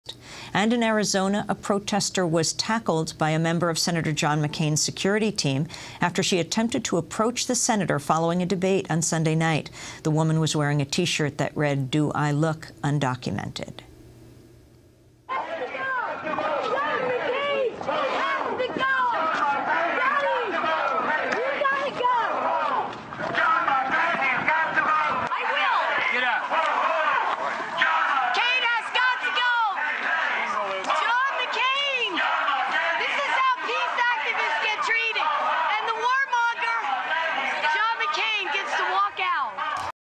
DN! Arizona Protester Lizzie Hourican Tackled by Sen. McCain's Security Team - Code Pink
Arizona Protester Tackled by Sen. McCain's Security Team

And in Arizona, a protester was tackled by a member of Senator John McCain's security team after she attempted to approach the senator following a debate on Sunday night. The woman was wearing a shirt that read "Do I look undocumented?"